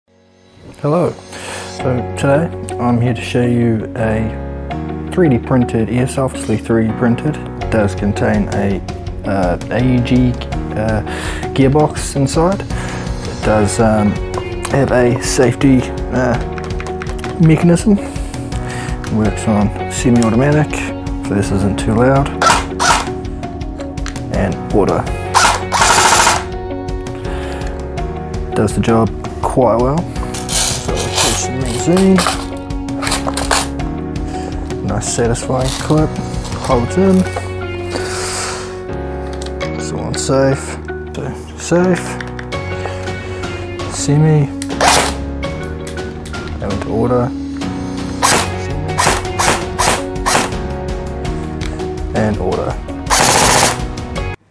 3D printed automatic airsoft
A brief overview of my 3d printed AEG.

For custom work visit goodcat3d.com.